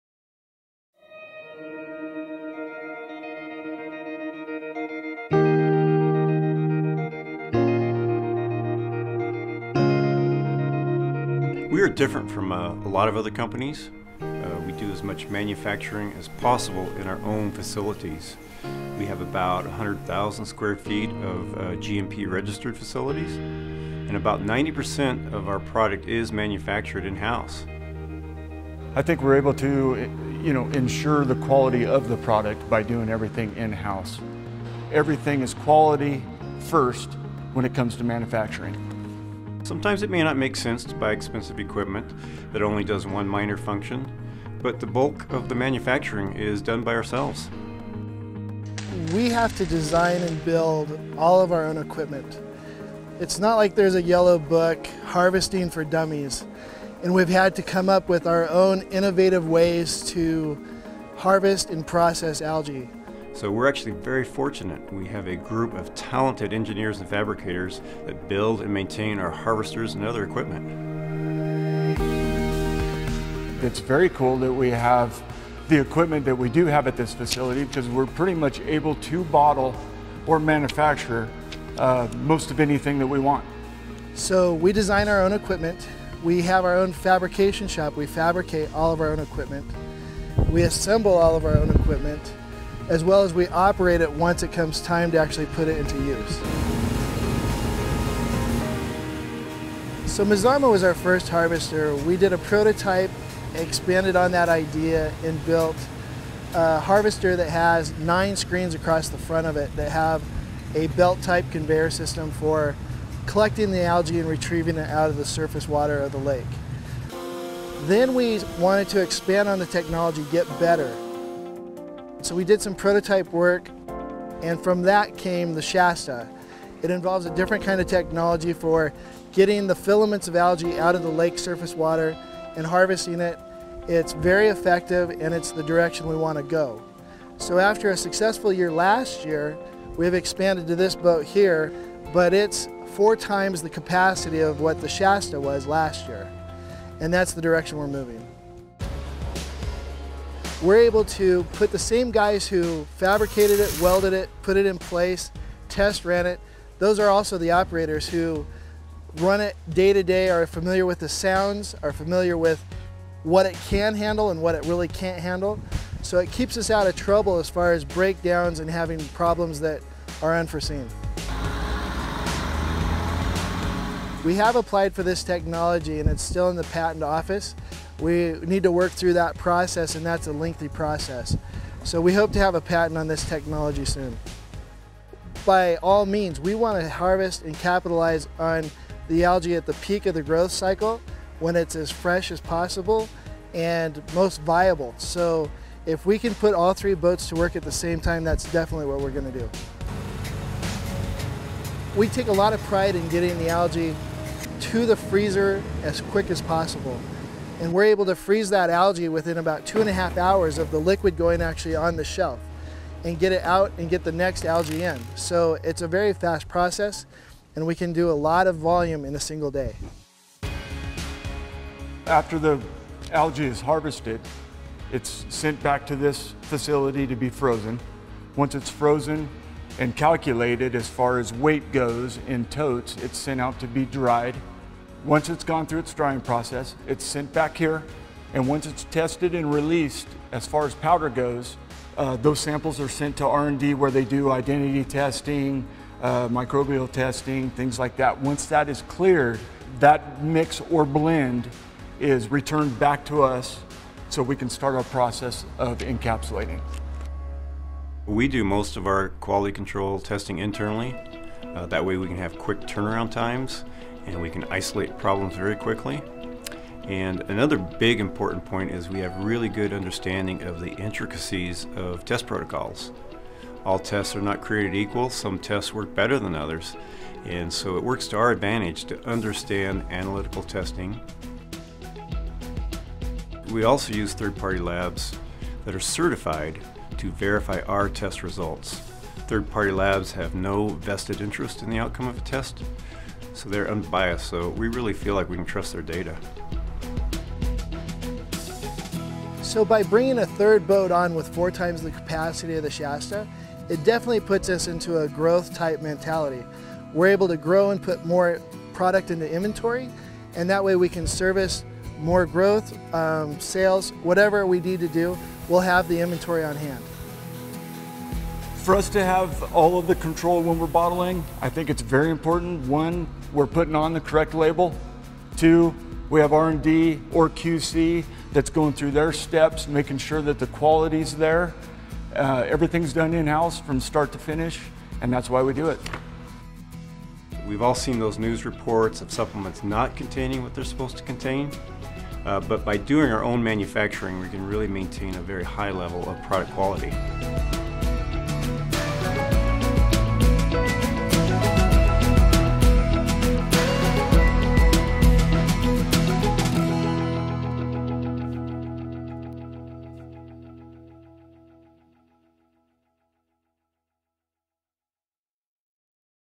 Algae is an Art: Our Manufacturing Processes
Our organic Wild Microalgae® is a fragile organism. It takes sophisticated technological know-how to harvest, cool, process, and dry in a way that preserves its delicate balance of nutrients. It goes from harvest to quick cooling to multiple cleanings to water removal to fast freezing and storage…all in a few hours! New Earth’s microalgae is wild harvested yearly and only grows in abundance in one place, Klamath Lake (Oregon). The lake is a unique ecosystem with headwaters located in an unspoiled mountainous region near Crater Lake National Park.